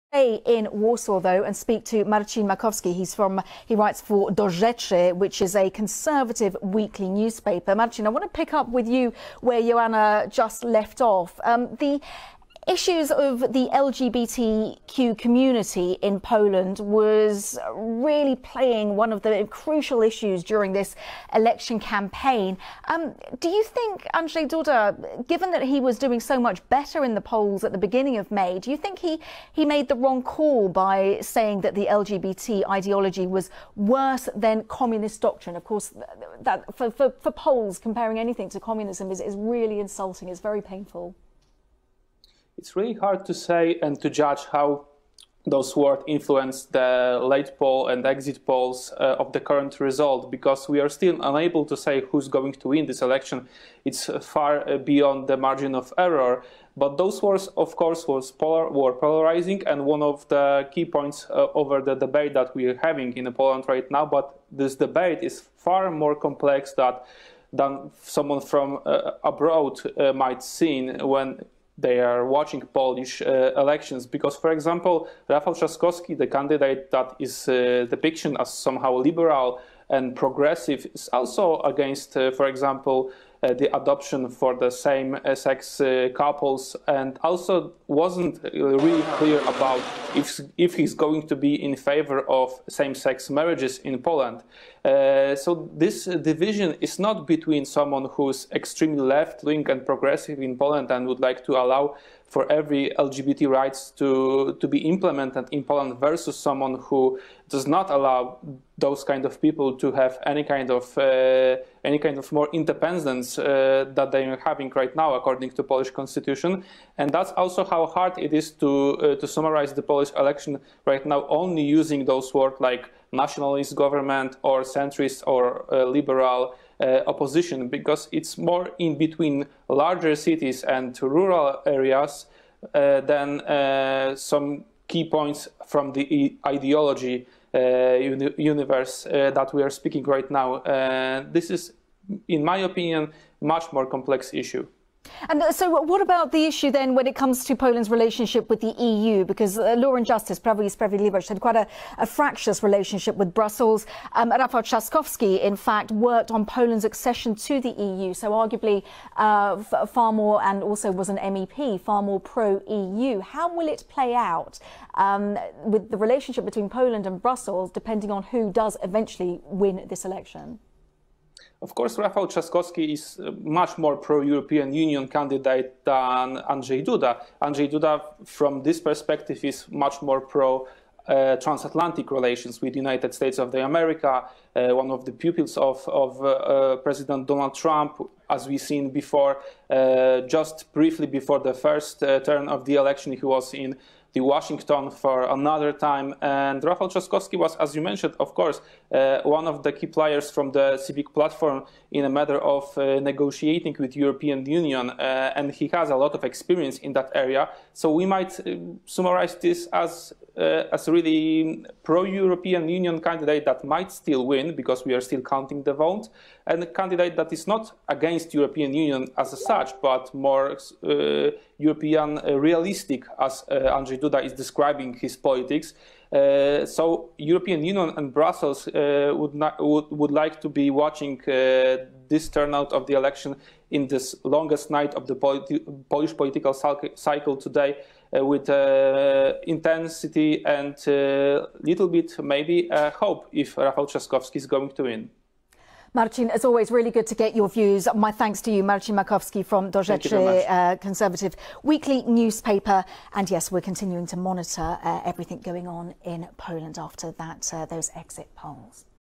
Marcin Makowski - BBC. Komentarz do II tury wyborów prezydenckich w Polsce
W jaki sposób tematyka LGBT oraz unijna wpłynęła na II turę wyborów prezydenckich w Polsce? Kto ostatecznie uzyska w nich zwycięstwo - Rafał Trzaskowski czy Andrzej Duda? O tym rozmawiałem z Katarzyną Maderą w BBC World News chwilę po wstępnych wynikach exit poll.